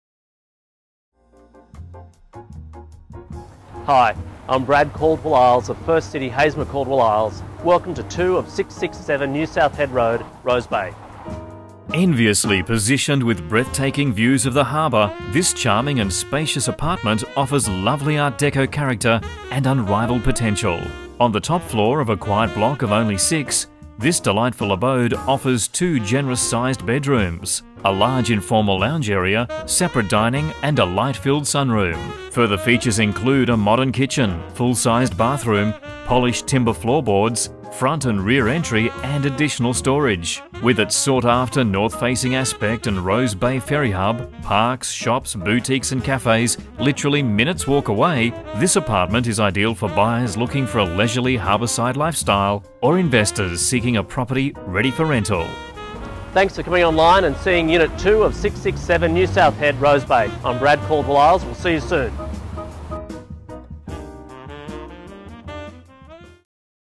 2/667 New South Head Road Rose Bay
Enviously positioned with breathtaking views of the harbour, this charming and spacious apartment offers lovely art deco character and unrivalled potential.

On the top-floor of a quiet block of only six, this delightful abode offers two generous sized bedrooms, a large informal lounge area, separate dining and a light-filled sunroom.

Further features include a modern kitchen,  full-size bathroom, polished timber floor boards, front and rear entry and additional storage.

With its sought-after north facing aspect and Rose Bay ferry hub, parks, shops, boutiques & cafes literally minutes walk away, this apartment is ideal for buyers looking for a leisurely harbourside lifestyle
or investors seeking a property ready for rental.